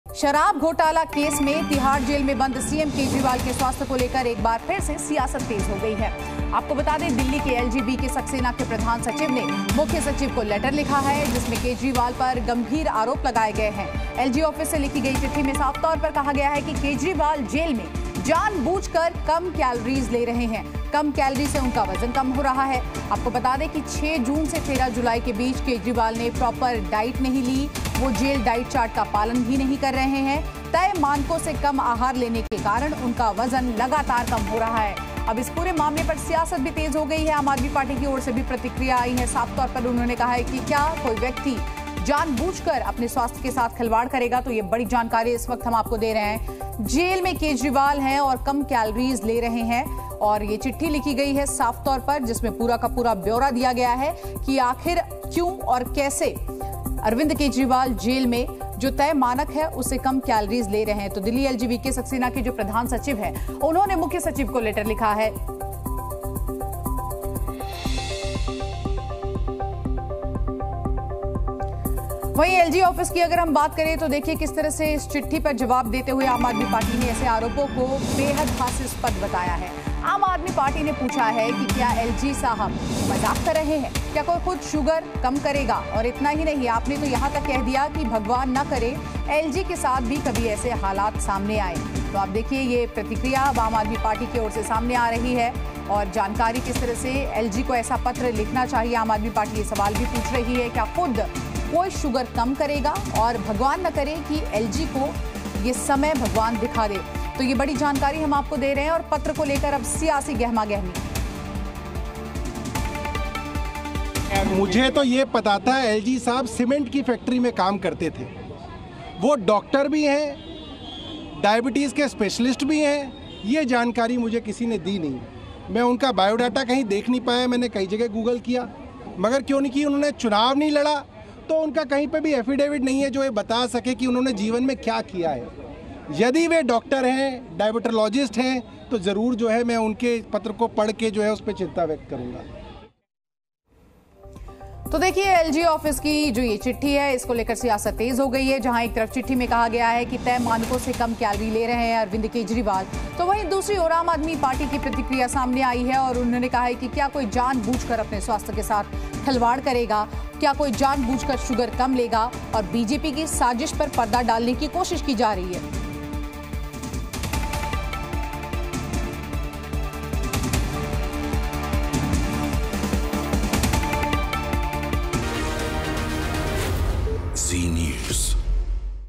Breaking : 'जेल में जानबूझकर कम कैलोरी वाली डाइट ले रहे सीएम केजरीवाल', LG दफ्तर ने लिखी चिट्ठी
शराब घोटाला केस में तिहाड़ जेल में बंद सीएम केजरीवाल के स्वास्थ्य को लेकर एक बार फिर सियासत तेज हो गई है...दिल्ली LG वीके सक्सेना के प्रधान सचिव ने मुख्य सचिव को लेटर लिखा है,,,जिसमें केजरीवाल पर आरोप लगाए गए हैं,,,,एलजी ऑफिस से लिखी चिट्ठी में कहा गया है कि केजरीवाल जेल में जानबूझ कर कम कैलोरी ले रहे हैं, कम कैलोरी से उनका वजन कम हो रहा है, 6 जून से 13 जुलाई के बीच केजरीवाल ने प्रॉपर डाइट नहीं ली, वो जेल डाइट चार्ट का पालन नहीं कर रहे। तय मानकों से कम आहार लेने के कारण उनका वजन कम हो रहा है.

Politics has once again intensified over the health of CM Kejriwal, who is lodged in Tihar Jail in the liquor scam case... The Principal Secretary of Delhi LG VK Saxena has written a letter to the Chief Secretary, in which allegations have been leveled against Kejriwal... In the letter written from the LG's office, it has been said that Kejriwal is deliberately taking less calories in the jail, his weight is decreasing due to less calories, Kejriwal did not take proper diet between 6 June and 13 July, he is not following the jail diet chart. His weight is decreasing due to taking less food than the set standards.